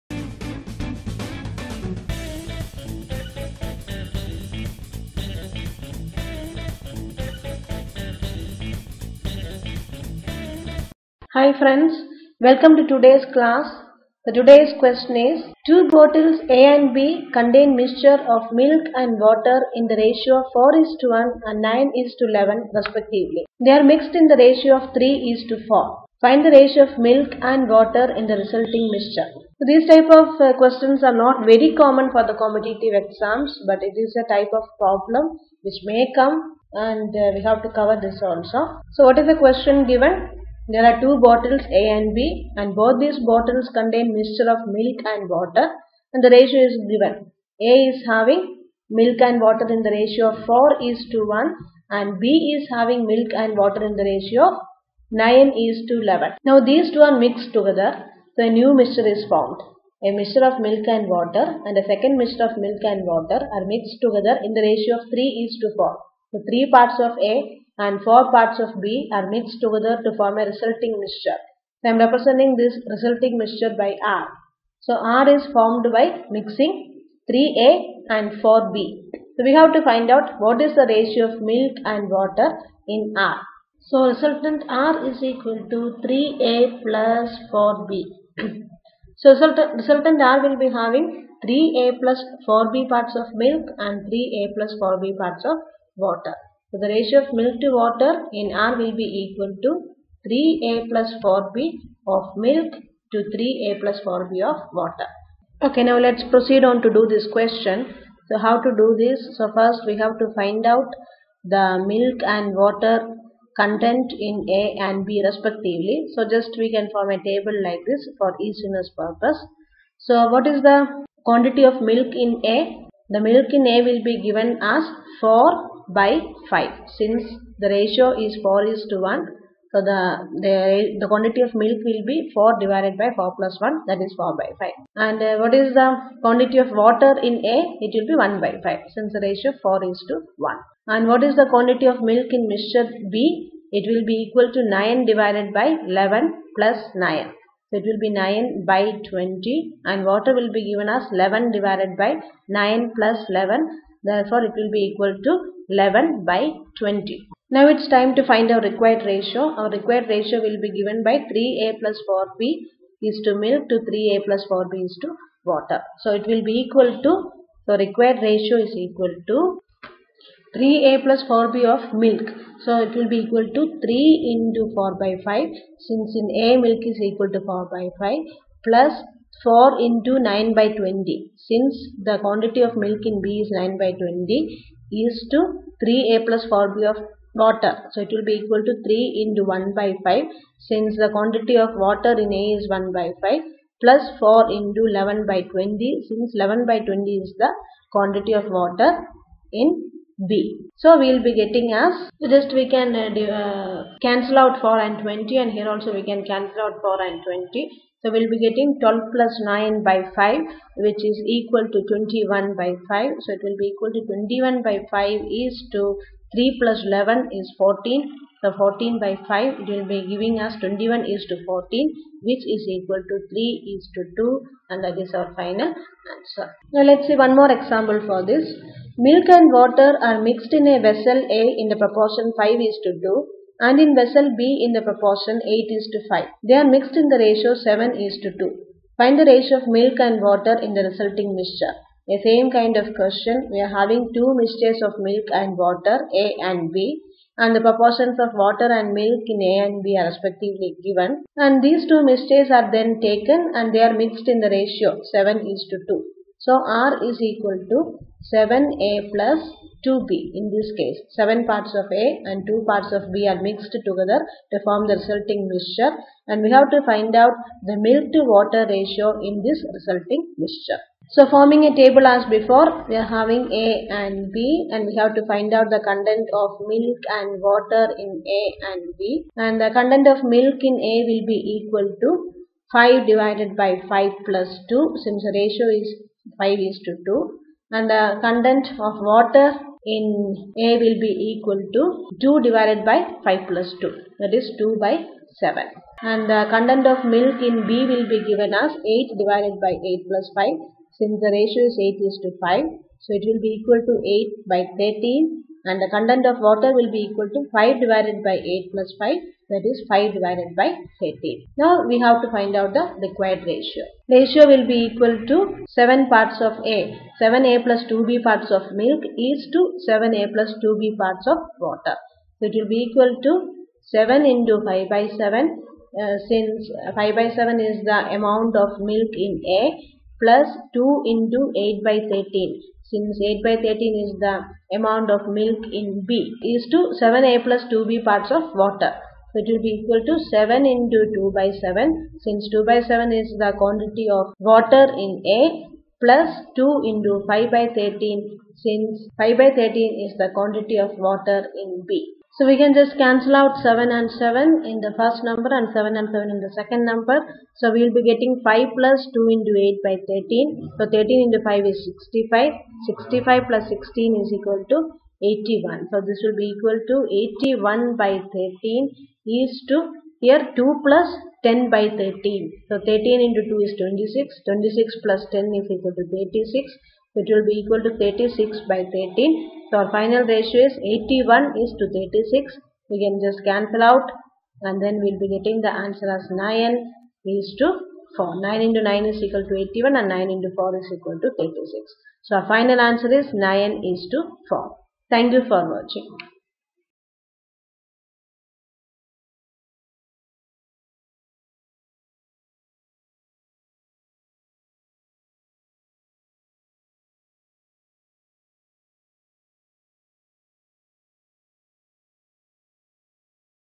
Alligation and Mixture | Mixing of Mixtures in a given ratio
This video shows how to solve Alligation and Mixture Questions related to Mixing of Mixtures in a given ratio